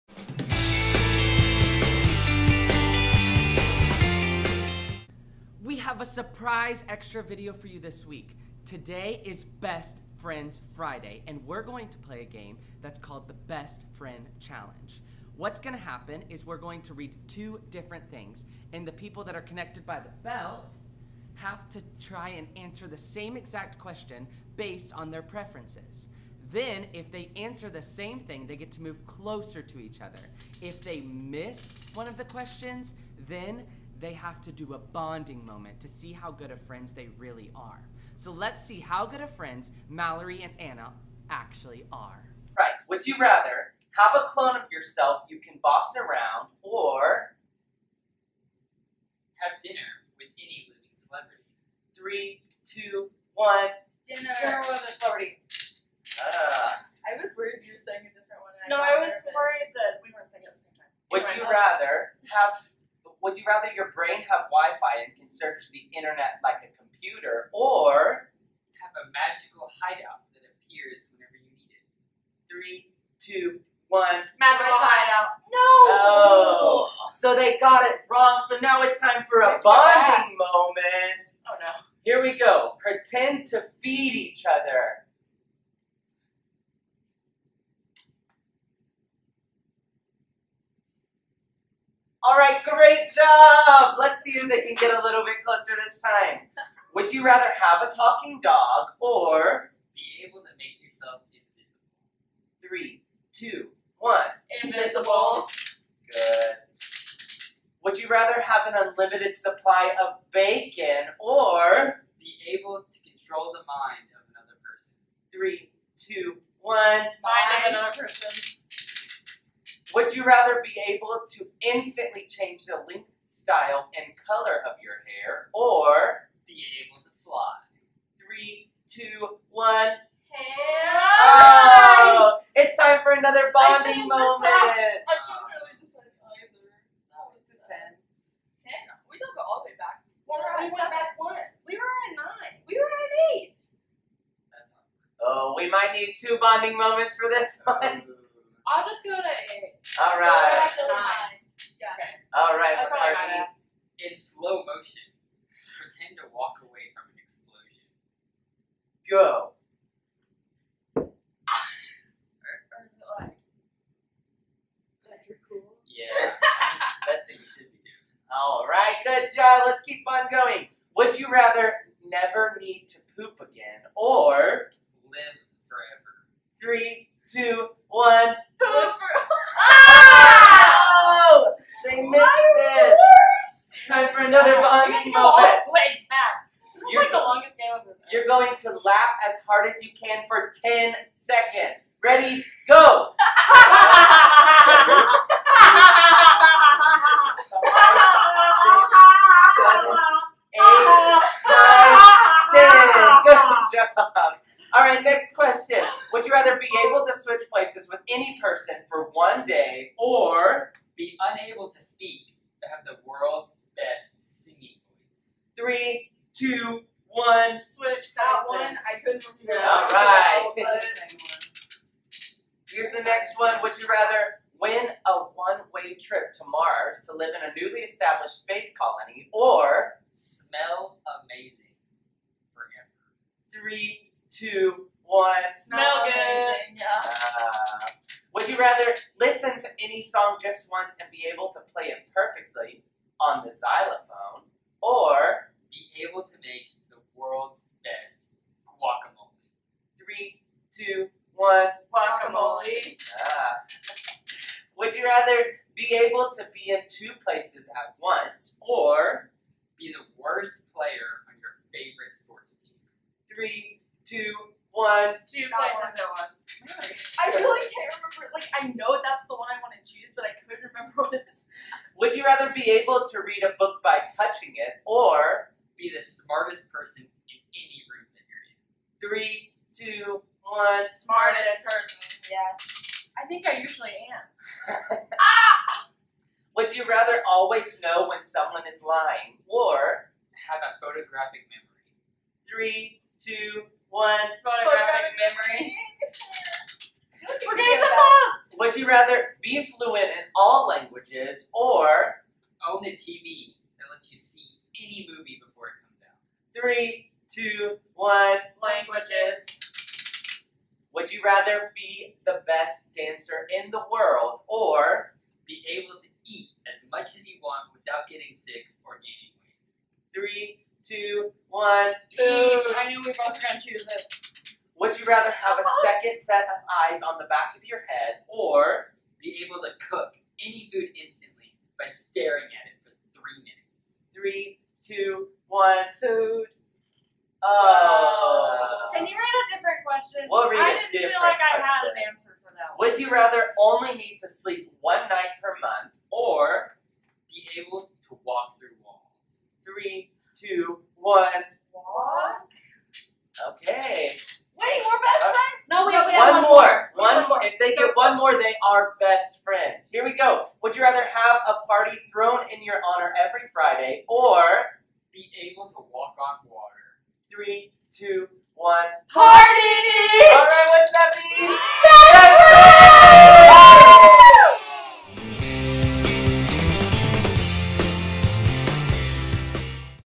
Best Friends Friday - Mallory and Anna
We have a special segment for you guys this week! Best Friends Friday! Tune in to see if Anna and Mallory are Best Friends!